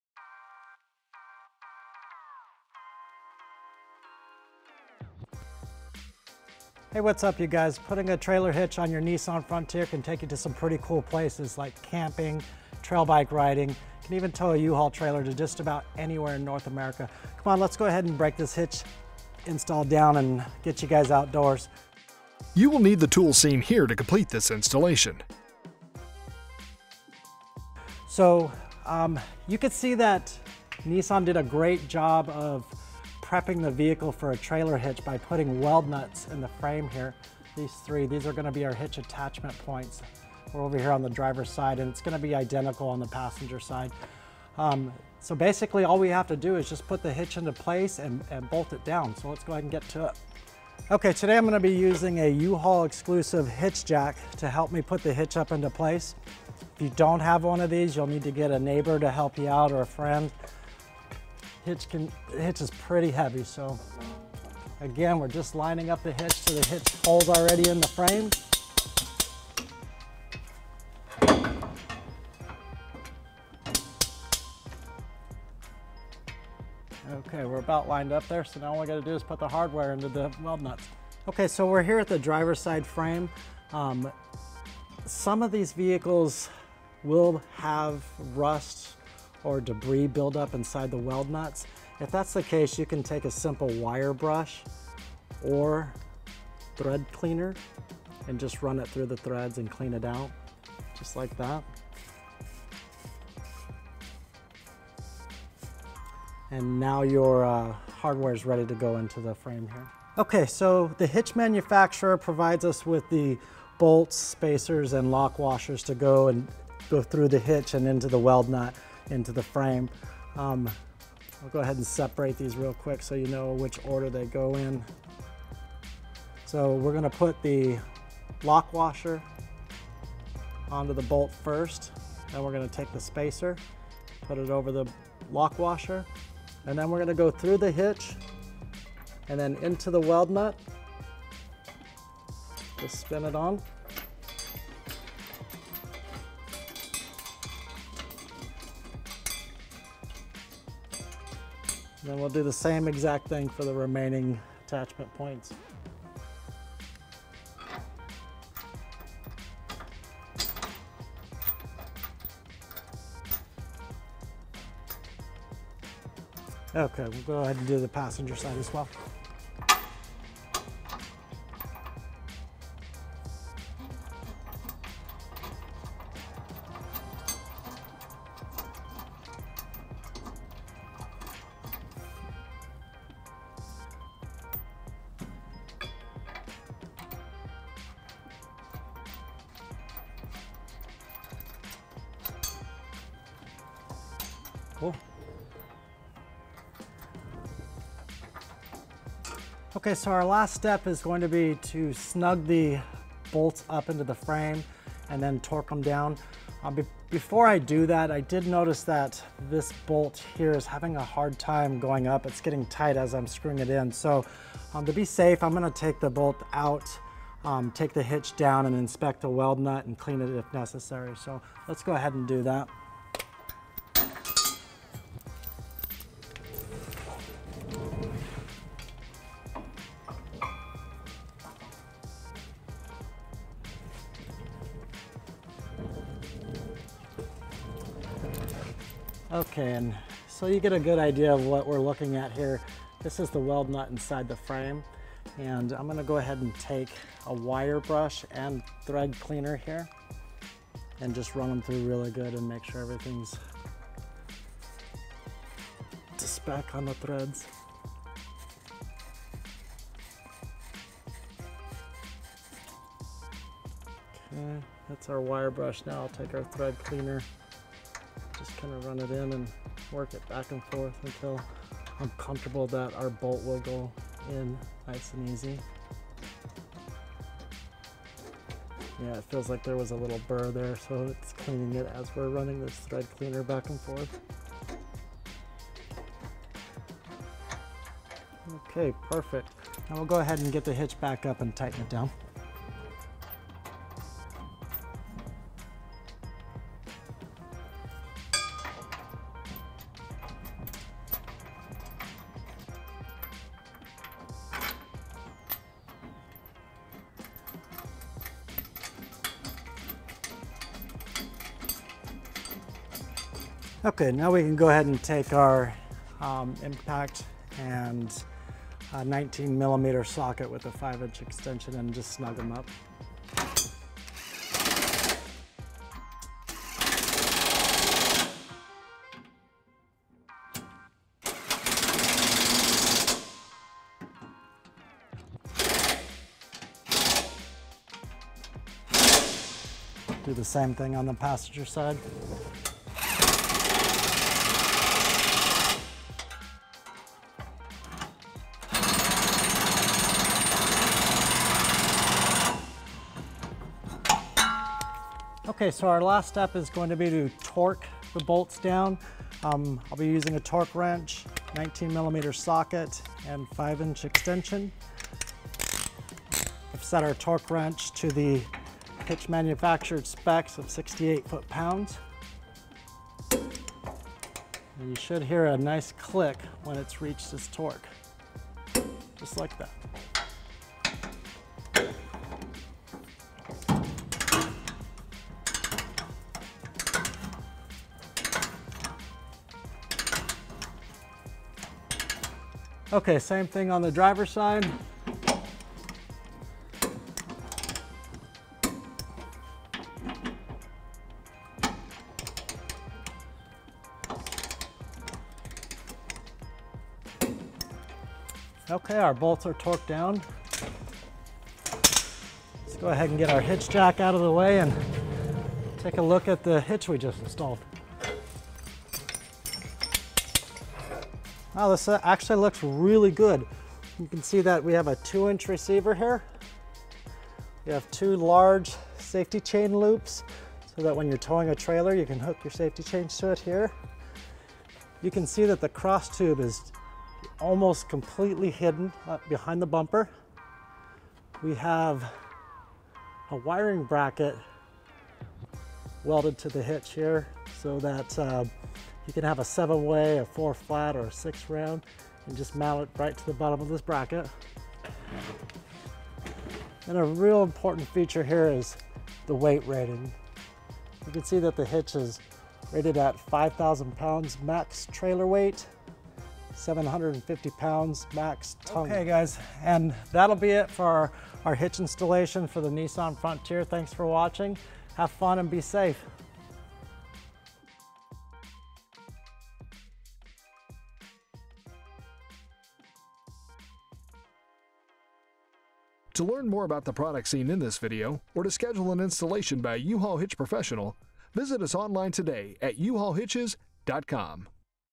2011 Nissan Frontier Trailer Hitch Installation
This video covers the trailer hitch installation for part number DRAW-TITE 75282 on a 2011 Nissan Frontier. Learn how to install your receiver on your Nissan Frontier from a U-Haul installation professional. With a few tools and our step-by-step instructions you can complete the installation of part number DRAW-TITE 75282.

To purchase this trailer hitch and other towing accessories for your 2011 Nissan Frontier,

0:00 Intro

0:22 Tool List

0:27 Raise hitch into place

1:50 Install hardware

3:20 Inspecting weld nuts

5:55 Torque bolts

7:01 Hitch details and measurements

8:06 Outro